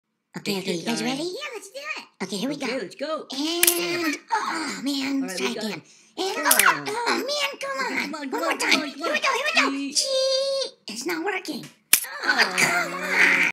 This drives me crazy!
I got this new camera for Christmas, but I don't think it works!

Tag your Stikbot videos with Stikbot, and we might include it on The Stikbot Show.